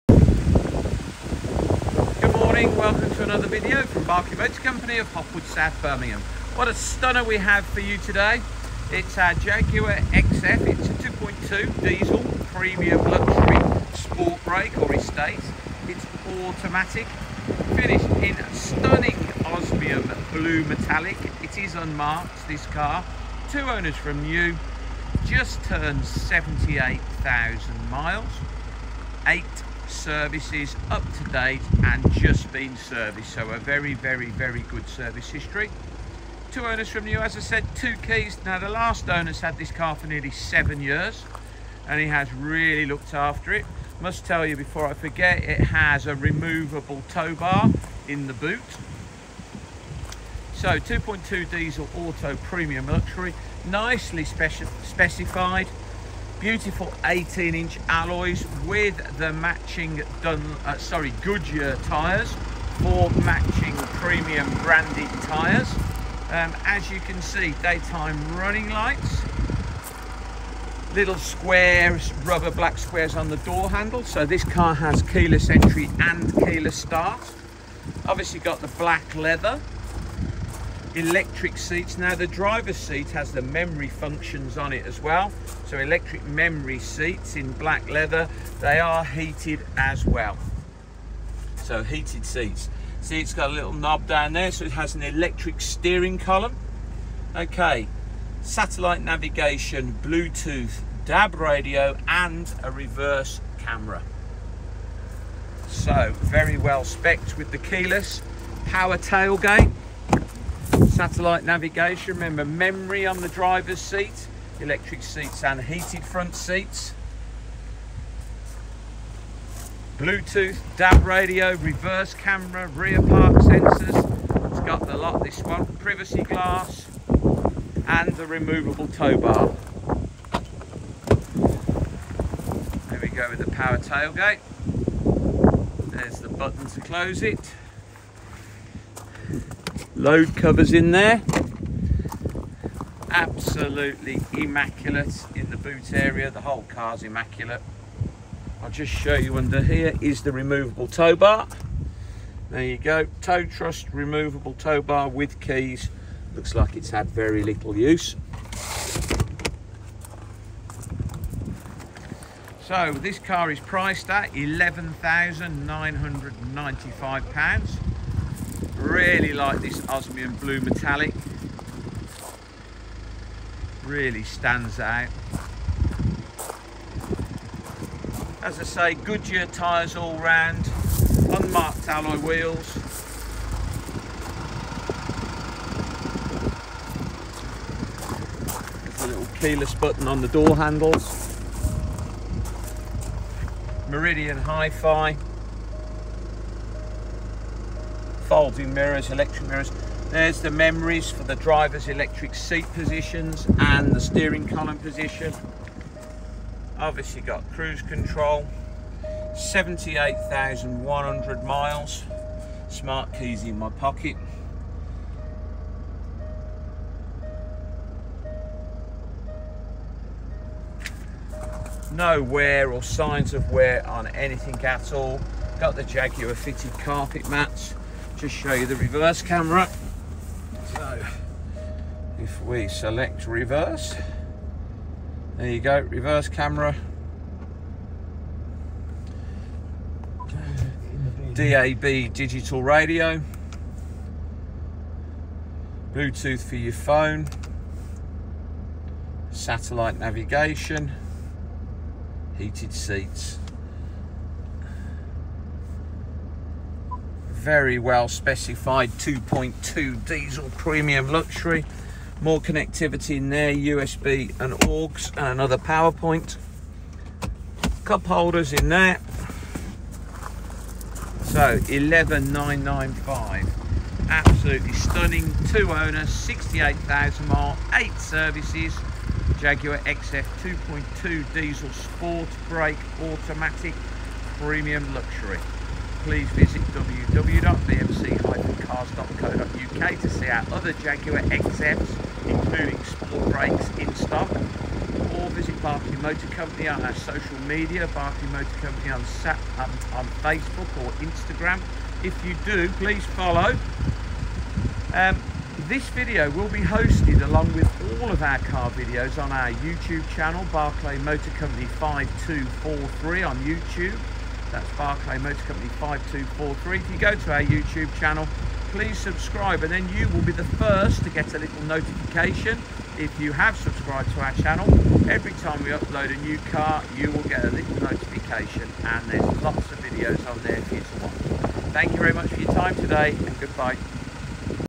Walk Through For Sale Jaguar XF 2.2d Premium Luxury Sportbrake Auto. OSMIUM BLUE! 8 Services!
78k miles. 8 Services Excellent Service History. Great Spec! Stunning Condition! 2 Owners! Last Owner for more than 6 years! £11995!‼️✅